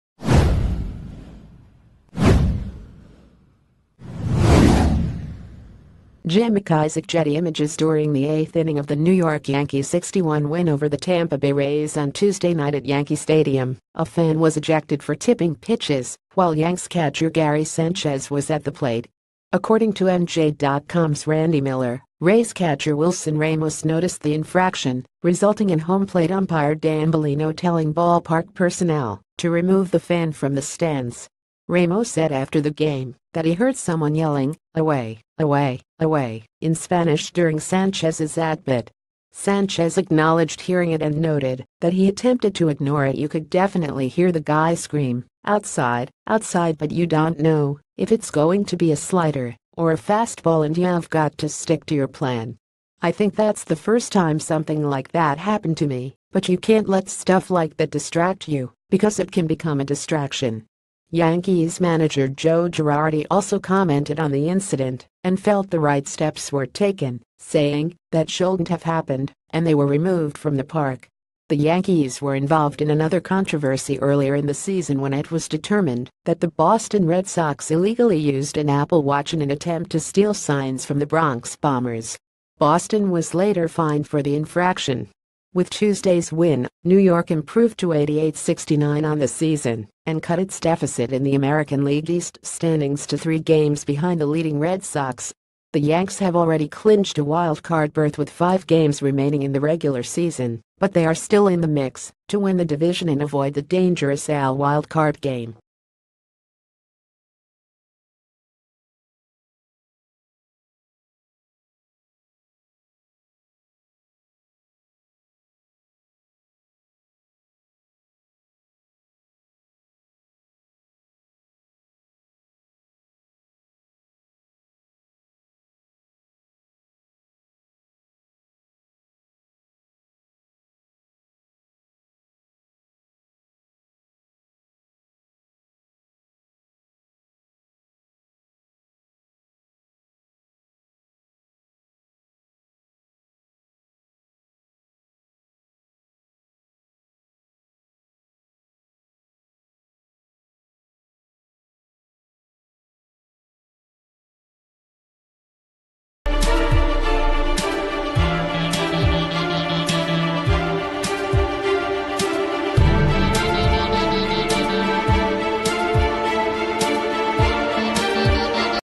Breaking News | Yankees fan ejected by umpire for tipping rays' pitches to gary sanchez
Yankees fan ejected by umpire for tipping rays' pitches to gary sanchez

Jim McIsaacGetty ImagesDuring the eighth inning of the New York Yankees 61 win over the Tampa Bay Ra...